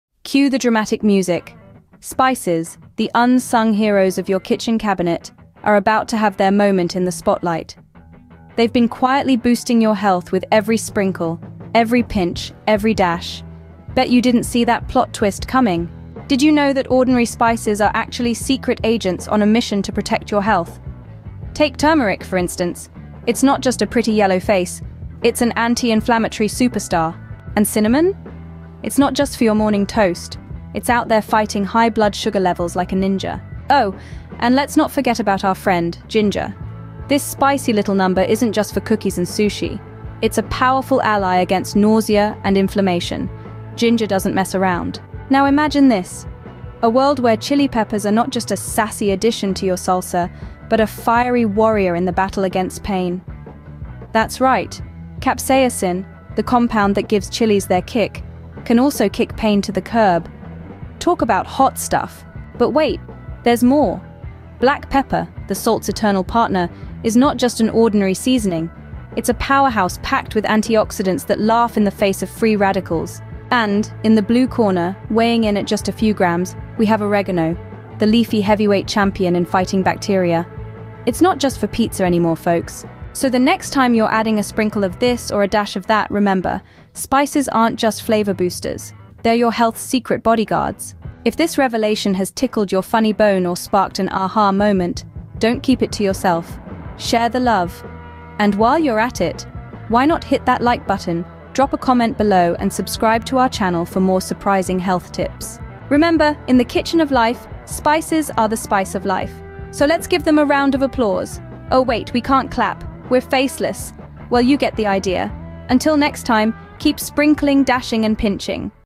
The SHOCKING BENEFITS of Spices On Your Health! Take One Teaspoon Of This
Did you know that spices have amazing benefits for your health, including healing leaky gut? In this video, we'll reveal how just one teaspoon of certain spices a day can improve your overall health and well-being. Say goodbye to inflammation and hello to a healthier gut!@PureBalanceInsights . Your health and wellness is our Priority. We love you.

OUTLINE:

00:00:00 The Unsung Heroes
00:00:16 Turmeric - The Anti-Inflammatory Superstar
00:00:27 Cinnamon - Fighting High Blood Sugar Levels
00:00:33 Ginger - A Powerful Ally
00:00:45 Chili Peppers - Fiery Warriors
00:01:02 Black Pepper - The Antioxidant Powerhouse
00:01:13 Oregano - The Bacteria Fighter
00:01:23 Spices - Your Health's Secret Bodyguards
00:01:31 Spread the Love
00:01:45 The Spice of Life